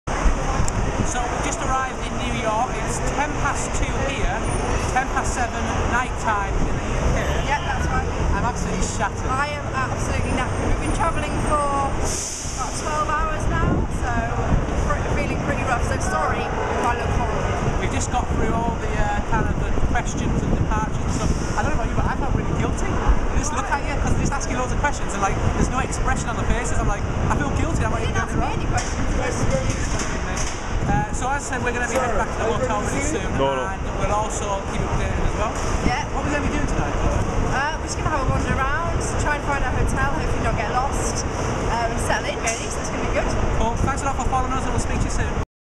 Spark Break America : Video 3 The Team arrive in NY!
Short video of Richie & Sarah talking about their journey to NY,